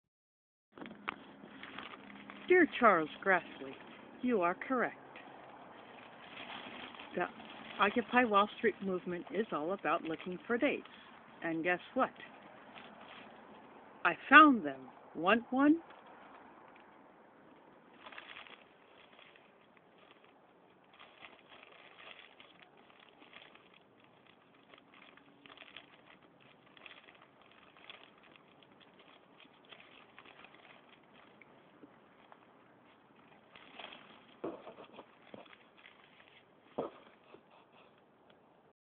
An Occupier Finds a DATE
In response to Senator Charles Grassley's statement Bout Occupy Wall Street supporters looming for dates, I show I found one!